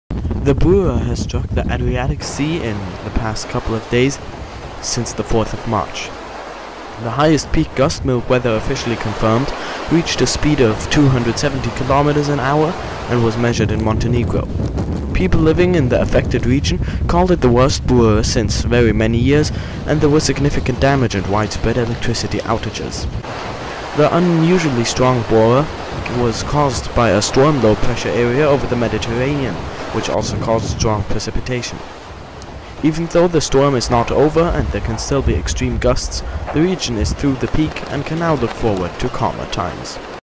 M.I.L.K. TV: Bura Storm in the Adriatic Sea / Croatia
The Bura has struck the Adriatic sea in the past couple of days, since the fourth of March. The highest peak gust MILK Weather officially confirmed reached a speed of 270km/h and was measured in Montenegro. People living in the affected region called it the worst Bura since very many years, and there was significant damage and wide-spread electricity outages. The unusually strong Bura was caused by a storm low-pressure area over the Mediterranean, which also caused strong precipitation. Even though the storm is not over and there can still be extreme gusts, the region is through the peak and can now look forward to calmer times.